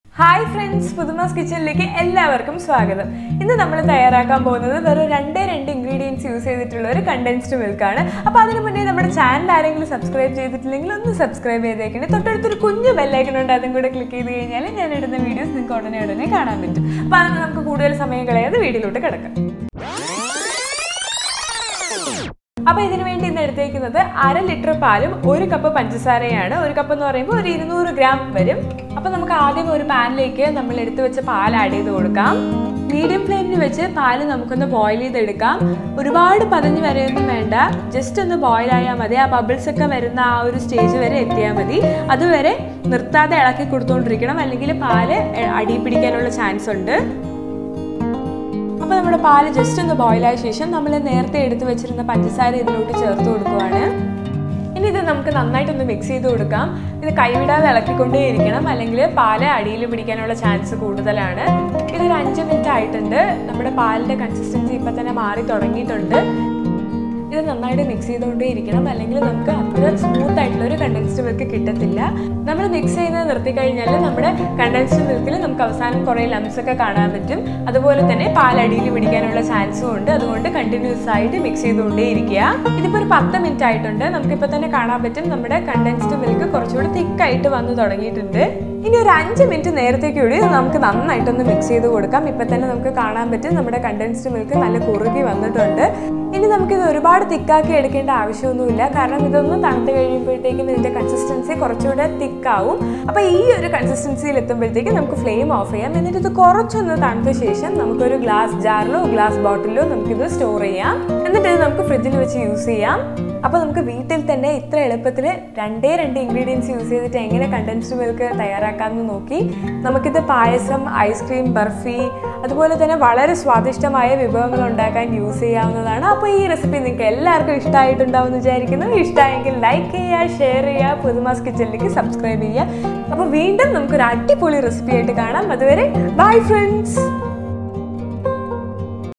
How to make Homemade Condensed Milk in Just 15 minutes? | Malayalam | with English Subtitles
Recipe by Manavumoni S

Hi friends! Hope everyone is doing good. Today we are making condensed milk using just 2 ingredients at home.
Condensed Milk or Popularly also known as Sweetened Condensed Milk or Mithai Mate or Milkmaid is used in several desserts such as Eggless Cakes, ice cream, payasam. Milk which is cooked with sugar for a long time brings in a smooth, thick creamy and delicious Condensed Milk. Although easily available, one can make it at home with 2 simple ingredients and at even less than half the price of a Condensed Milk we buy at stores

Ingredients for making creamy Condensed Milk

1. Milk - 1/2L
2. Sugar - 1cup(200gm)

Try this yummy Condensed Milk at home and let us know your feedback.

Have a healthy life!